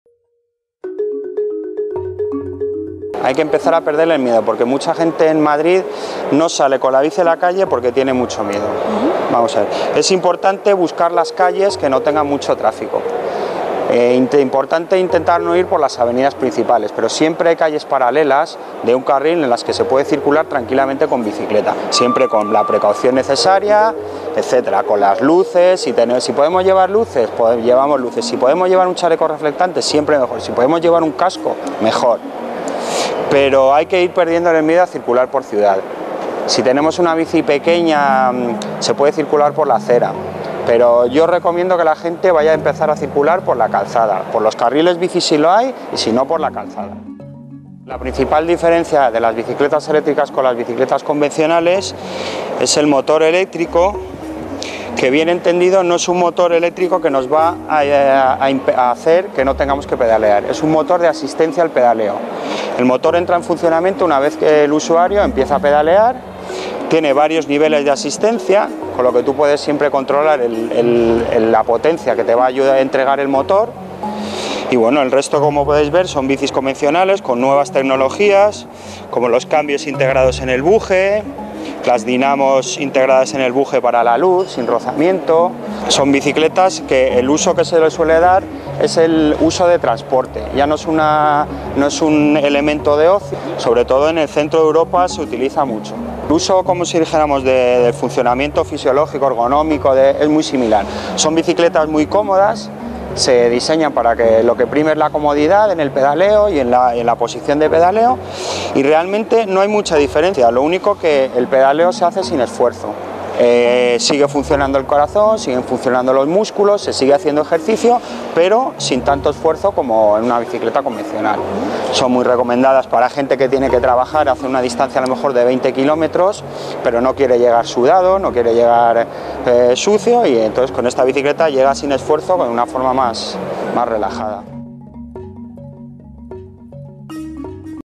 Consejos para ir en bici por la ciudad
Cada vez son más las familias que  se preocupan por llevar una vida sana y hacer ejercicio. Ignacio Folgueira, de la empresa By Bike, nos explica qué tener en cuenta para ir en bici por la ciudad y nos habla de las bicis eléctricas, una nueva forma de transporte que se está extendiendo mucho.l